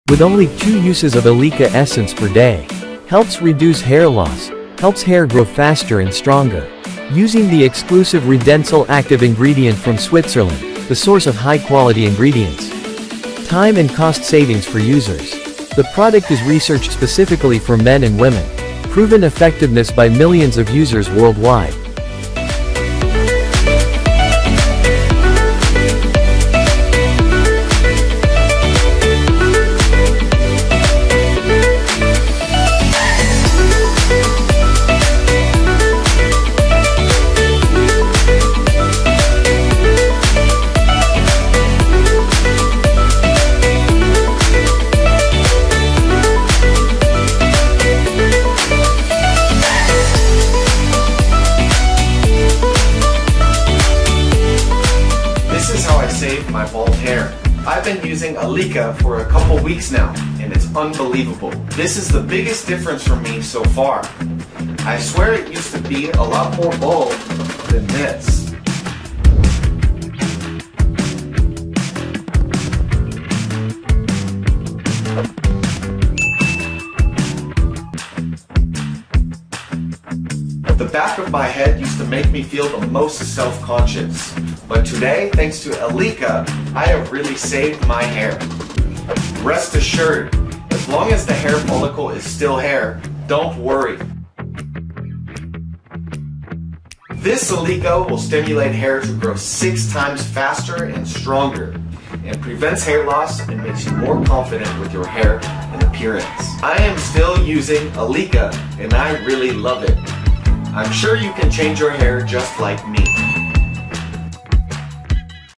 Good News for men suffering from hair loss, thinning hair, baldness....
ALIKA is a breakthrough solution for hair restoration with high-tech technology, featuring a trio of clinically proven hair loss treatment ingredients - Redensyl®, Capixyl™, and Baicapil™ (RCB). Regrow hair in just 8 weeks. Enjoy thicker, fuller, and stronger hair while preventing further hair loss.

👉 Join the thousands of satisfied customers around the world who have already transformed their hair and their lives with ALIKA